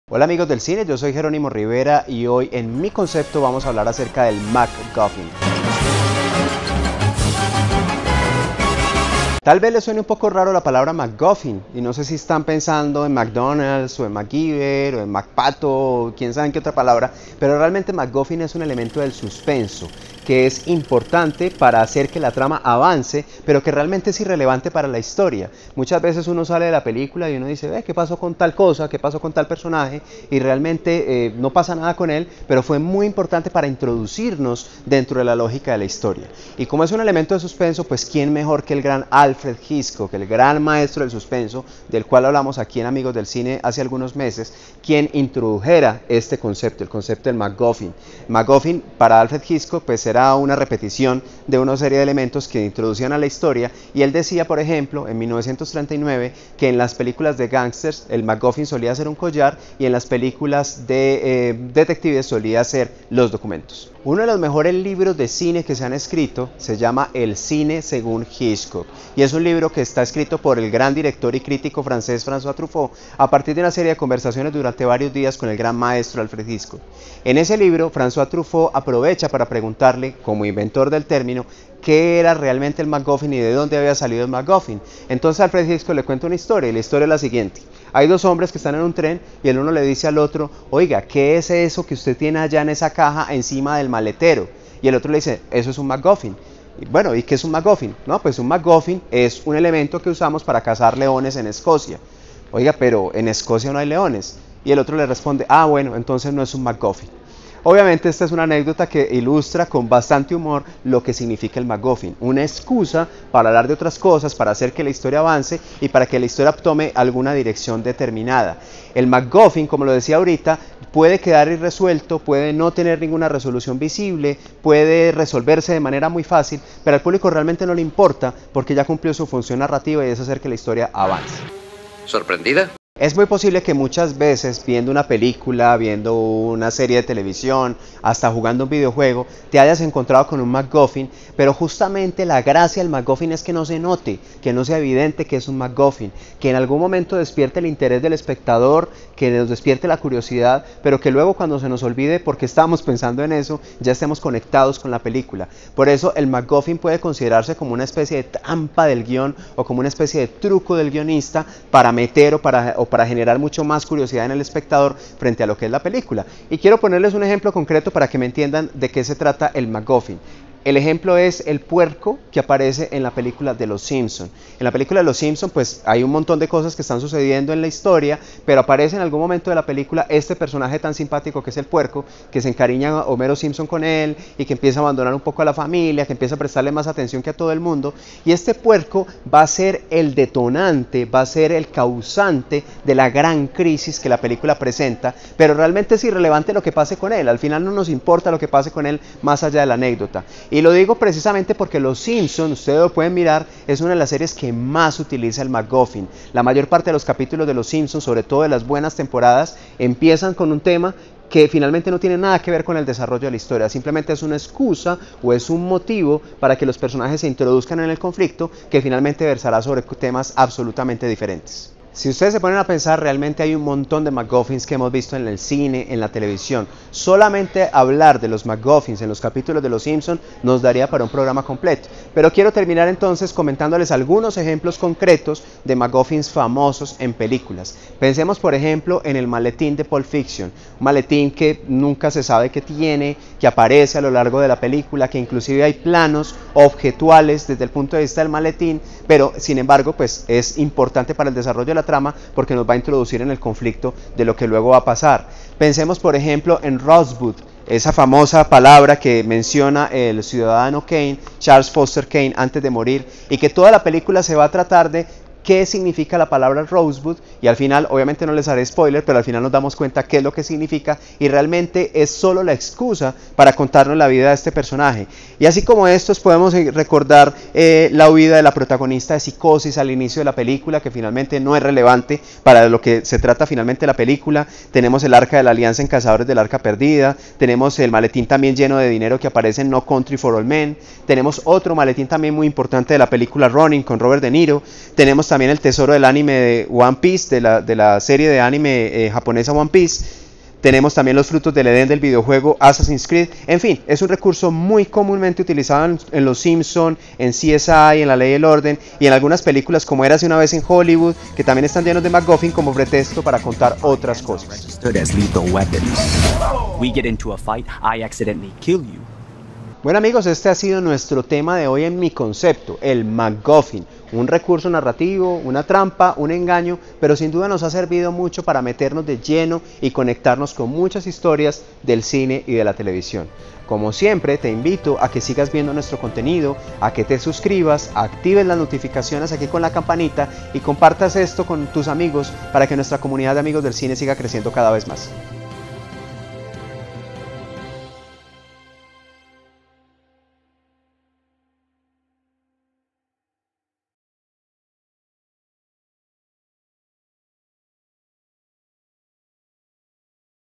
McGuffin: Eficaz recurso narrativo
El McGuffin es un eficaz recurso narrativo, consistente en usar un personaje, tema o conflicto que permite avanzar la acción pero que, a la larga, no es relevante para la historia. En nuestra sección "Mi concepto" te damos algunos ejemplos y te comentamos más sobre este truco de los guionistas.

Amigos del cine es una iniciativa con propósitos netamente educativos.